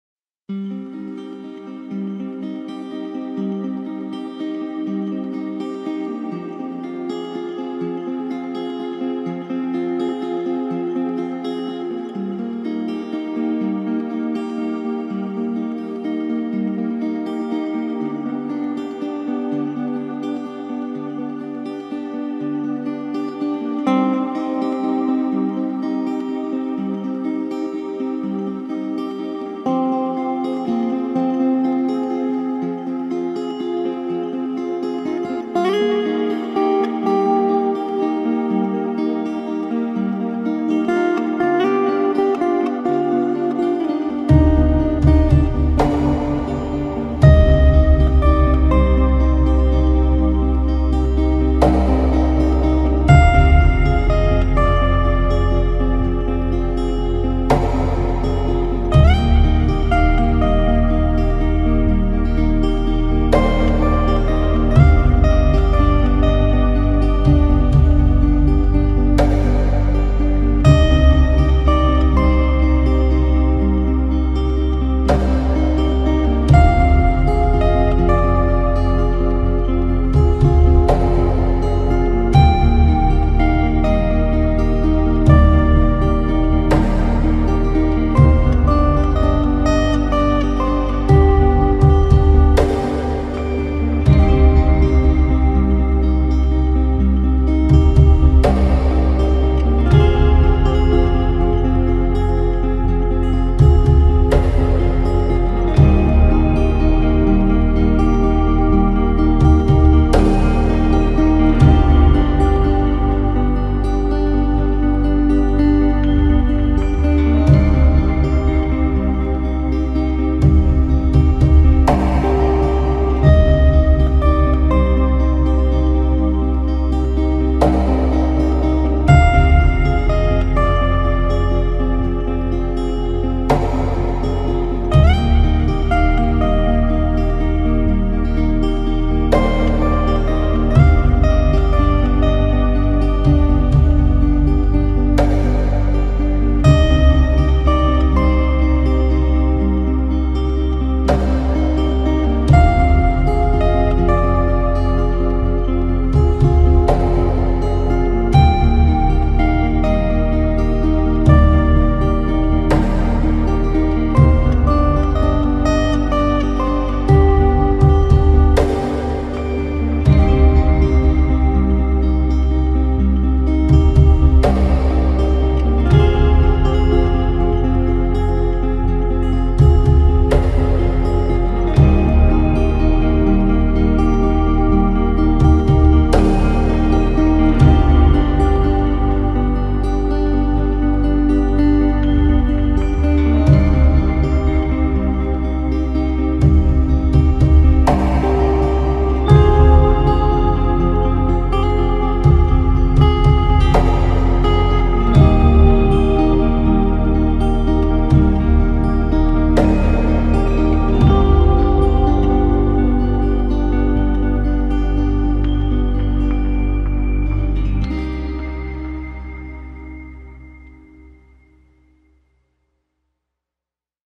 Halinów z drona.
Kilka ujęć Halinowa  z lotu ptaka. Jesień 2023.